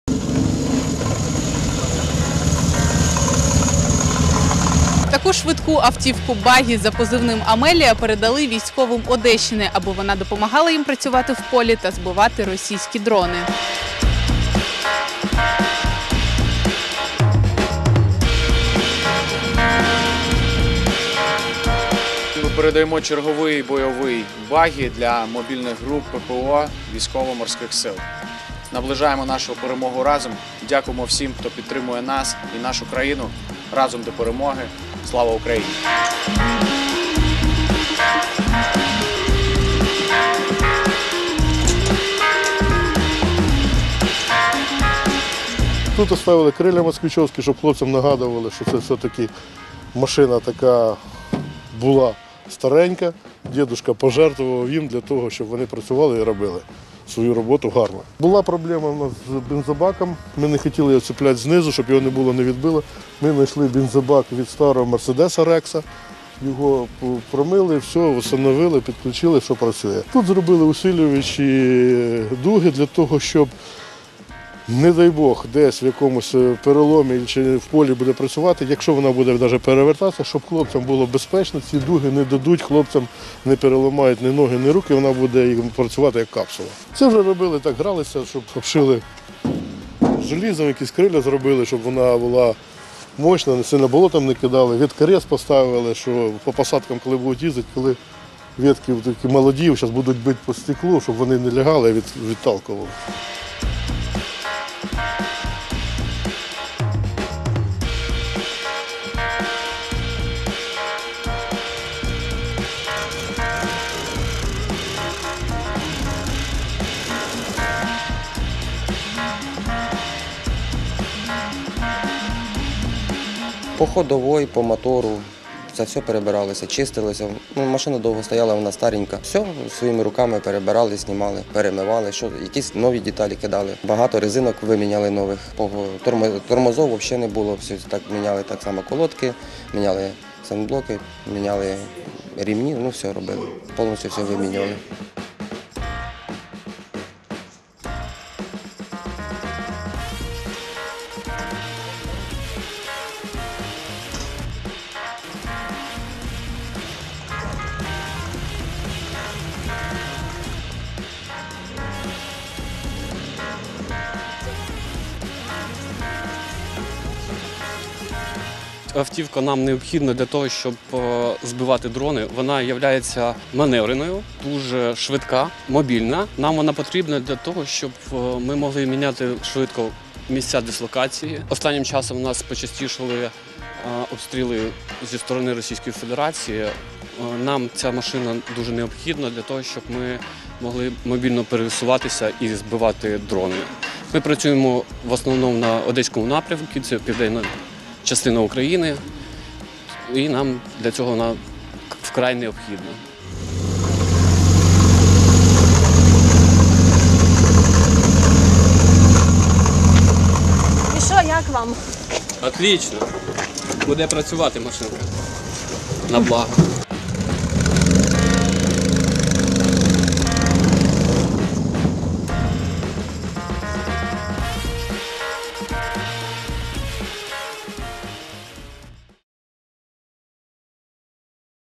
Співак Артем Пивоваров передав військовим в Одесі багі, щоб збивати дрони
Бойову автівку-багі одеські волонтери передали мобільним групам ППО військово-морських сил України. Її впродовж 2 тижнів збирали працівники ГО Майстерня незламності. До передачі автівки долучився і співак Артем Пивоваров, він допомагав волонтерам зібрати кошти на автівку.

Кореспондентка — Влада Матвійчук, оператор — Дмитро Бондаренко.